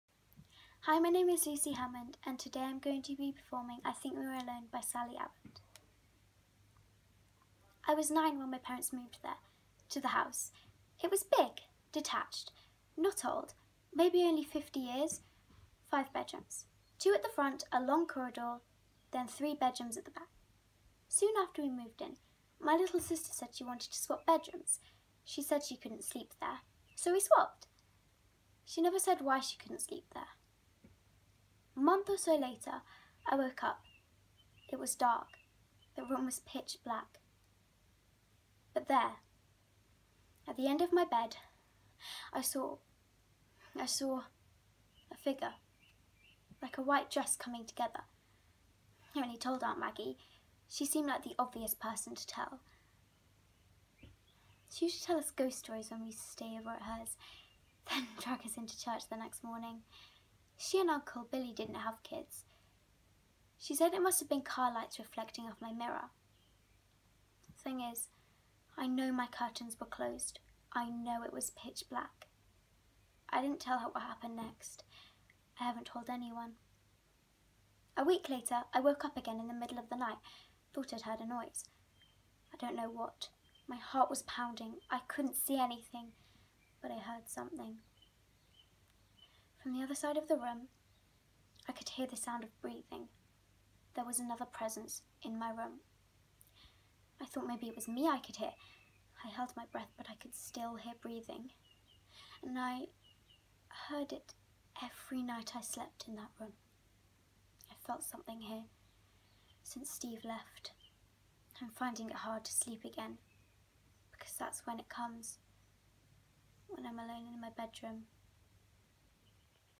Monologue from I Think We're Alone by Sally Abbott
First Year Drama Scholar Lucy shares a monologue she prepared as part of our TheShowMustGoOnline initiative. During the lock down period Performing Arts pupils at Bede's Senior School continued to enjoy opportunities to engage in a challenging and enriching programme of Drama education through our remote learning platforms.